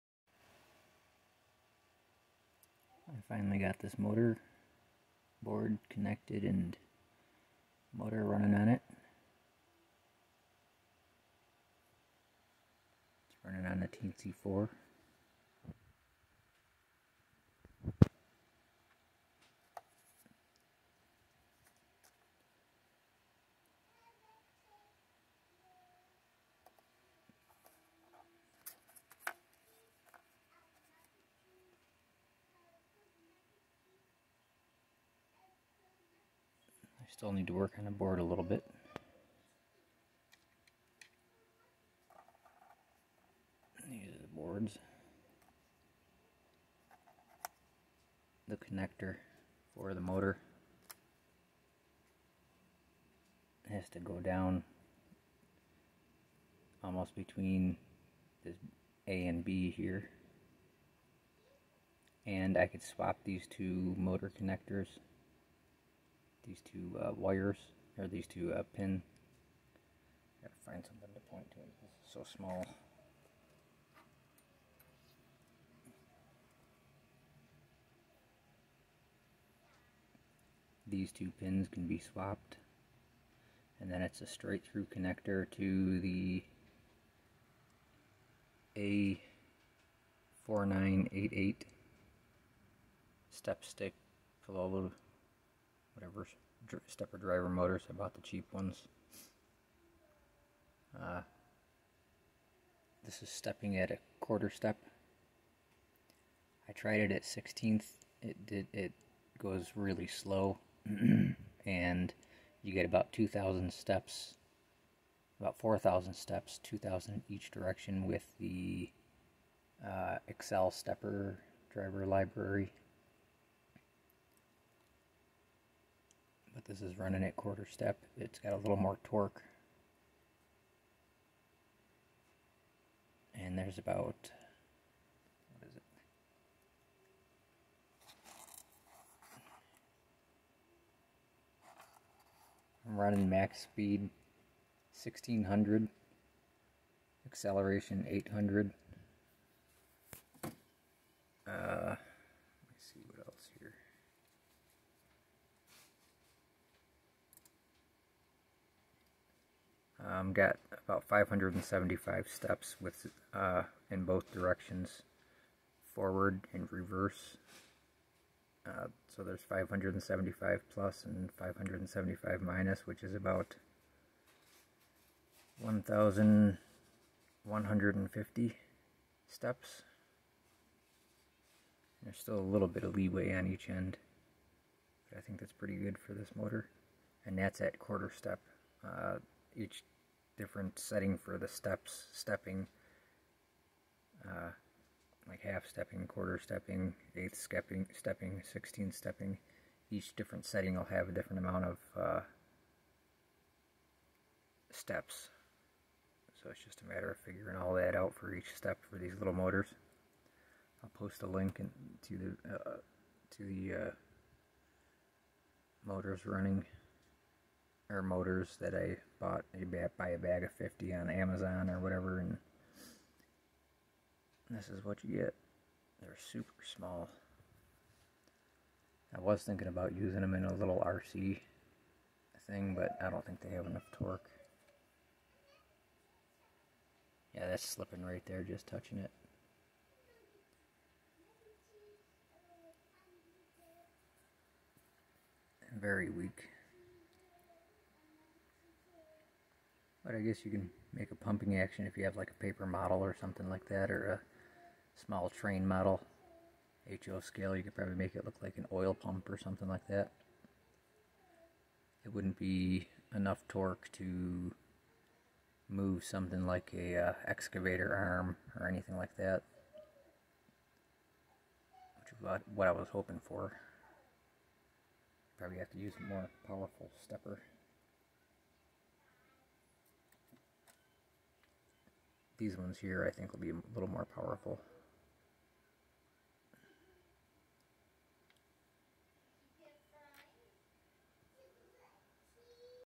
Micro Stepper Motor on custom pcb A4988 teensy 4.0
I made a custom PCB for some Micro Stepper Motors I bought off of Amazon. The custom PCB needs a little more work but they work as designed. Teensy 4.0 is used to control four of these motors using an A4988 driver.

These motors have 25 Ohm coils.  I have found some that look exactly the same with 12 Ohm coils.  The 25 Ohm coils are being run at 12V  - 0.200 Amp.  Very week!  I can probably increase to current a bit since the motors are not getting even remotely warm.

Software - Requires "AccelStepper" library.  Samples from the lib work fine.